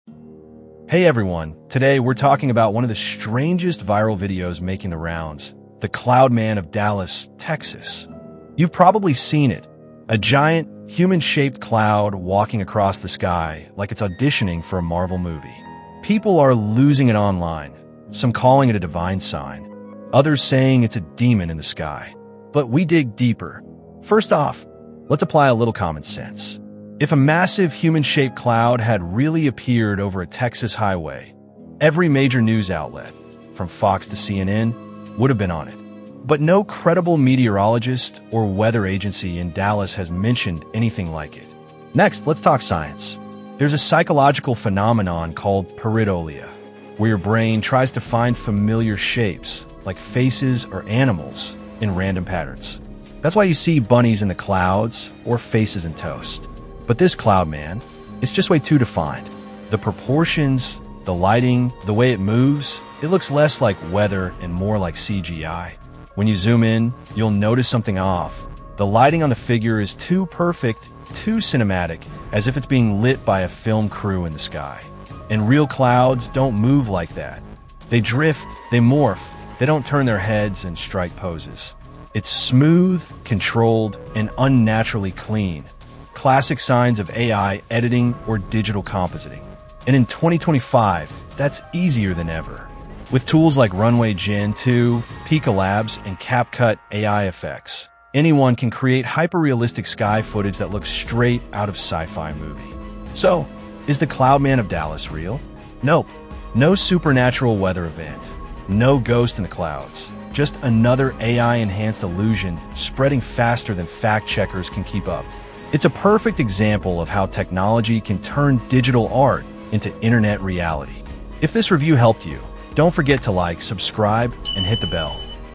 Is the Cloud Man of Dallas Texas Real? Viral TikTok Mystery Debunked!
Is the viral “Cloud Man of Dallas, Texas” video real or just another AI hoax? Millions of people are sharing footage showing a human-shaped cloud walking across the sky — but no credible source has confirmed it. In this exposé, we break down the video frame by frame, explain the science behind pareidolia, and reveal the AI editing tools likely used to create it. Watch till the end to find out how easily fake visuals spread online and how to spot them before they fool you.